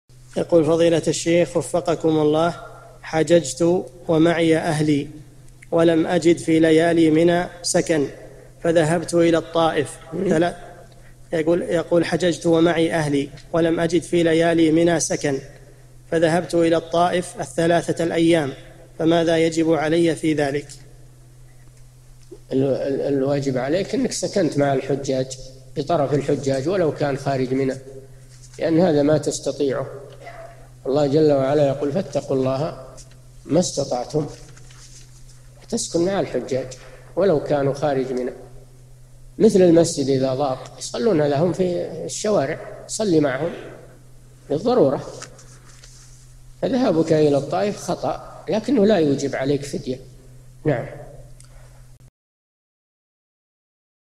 [405 /1350] ذهبت للحج معي أهلي، ولم أجد في ليالي منى سكن، فذهبت إلى الطائف الثلاثة الأيام..؟
-[405 /1350] السؤال: حججت ومعي أهلي، ولم أجد في ليالي منى سكنٍ، فذهبت إلى الطائف الثلاثة الأيام، فماذا يجب علي في ذلك؟
 الجواب: الواجب عليك إنك سكنت مع الحجاج، بطرف الحجاج ولو كان خارج منى؛ لأن هذا ما تستطيعه، الله تعالى يقول: ﴿فَاتَّقُوا اللَّهَ مَا استَطَعْتُمْ﴾ [التغابن: 16] تسكن مع الحجاج، ولو كانوا خارج منى، مثل المسجد إذا ضاع يصلون في الشوارع، صلِّ معهم للضرورة، فذهابك إلى الطائف خطأ، لكنه لا يُوجب عليك فدية.
للاشتراك في قناة الشيخ صالح الفوزان
أسئلة وأجوبة من شرح قاعدة جليلة ( 1350 ) مقطع

للاستفسار والإقتراحات والدعم
رئيس الفريق
لمشروع كبار العلماء